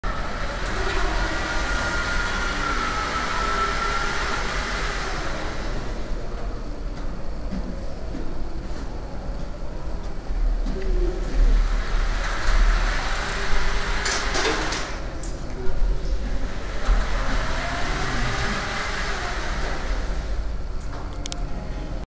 Walking the light at London's V and A tunnel entrance London Design Festival 2012